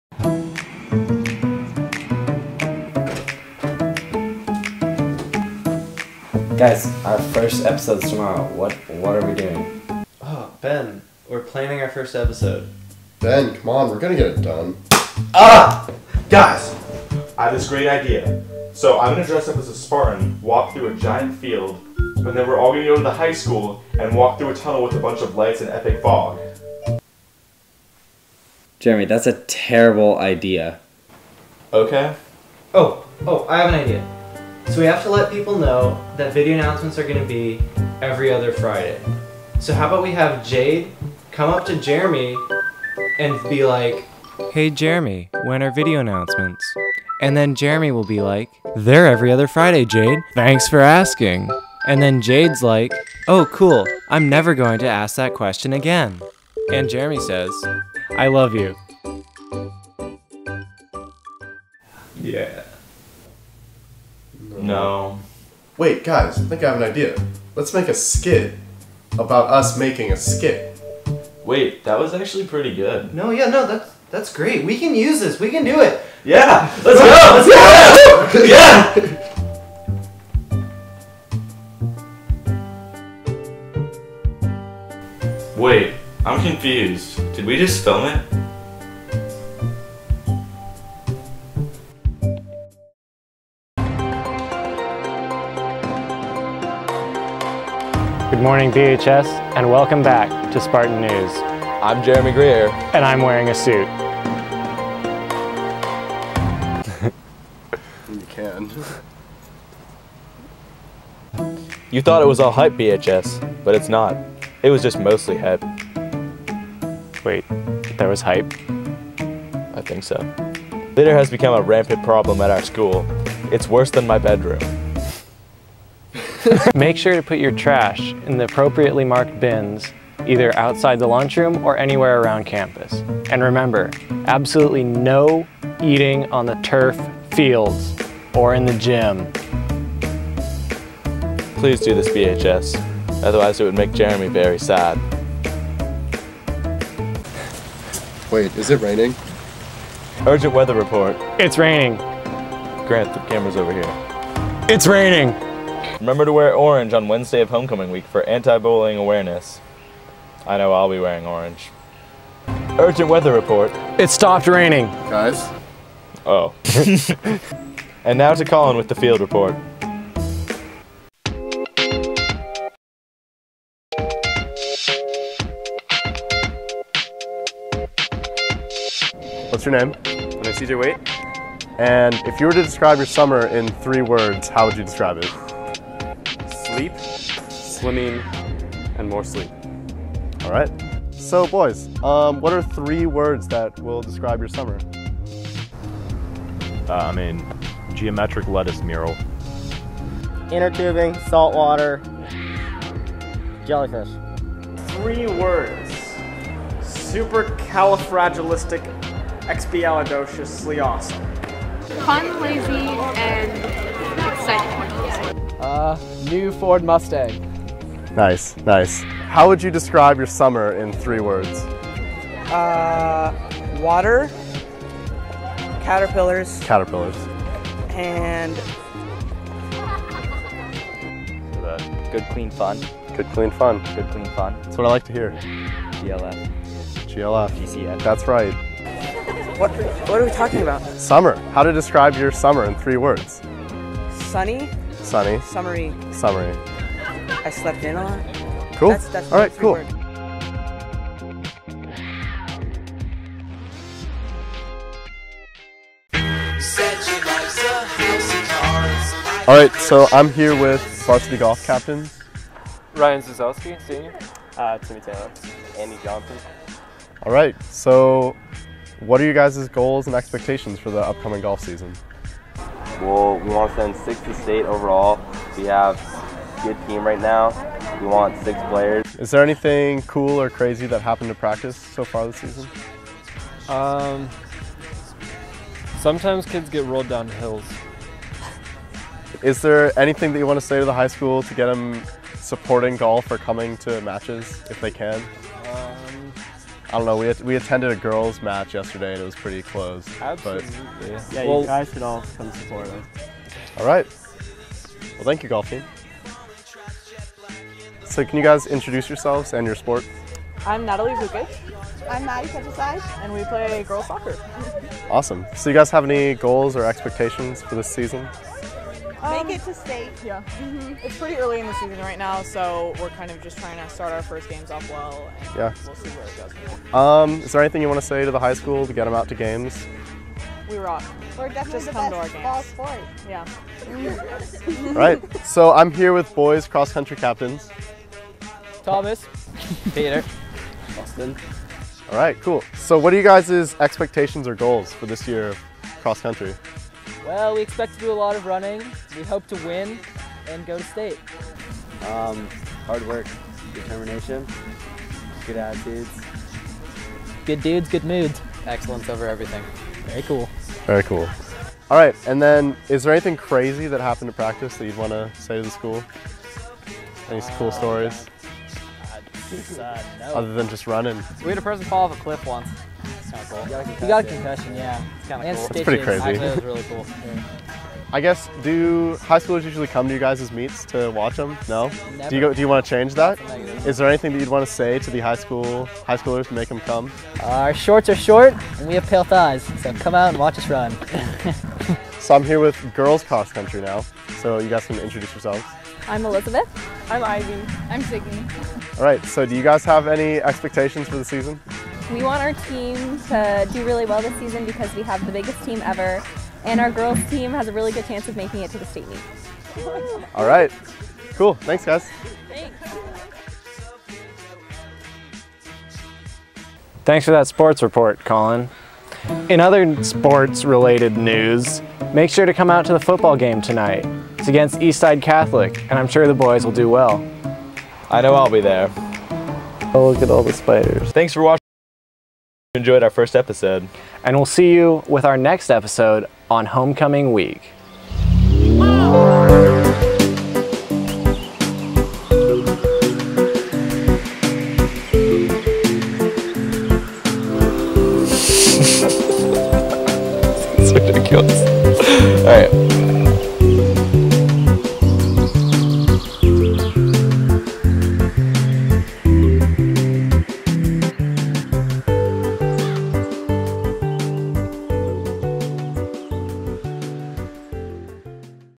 Spartan News - Episode 1
The return of Friday video announcements, Spartan News is a collaboration between Colin Costello, Ben Cowan, Jeremy Greer, and Grant Dalton for the Bainbridge High School community. Enjoy the first episode for the week of September 27th, 2013.

ANCHORS
Jeremy Greer
Grant Dalton

FIELD REPORTER
Colin Costello

PRODUCED BY
Ben Cowan
Colin Costello